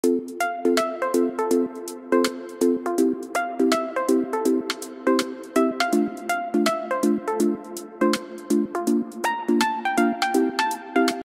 Meet My Newest Crocheted Sea Creature!
Beluga whale pattern coming to my shop soon! 🌊

You can find all of my crochet patterns here: theresascrochetshop.com

Musician: @iksonmusic